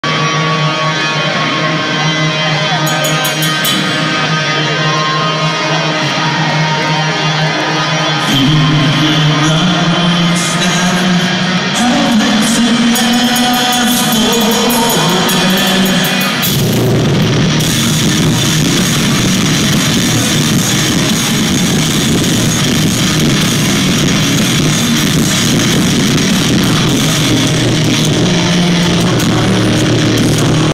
breakingbenjamin
at Blossom Music Center,Cuyahoga Falls,OH 8/27/23

(They were the loudest out of the 2 bands,they video may not sound right)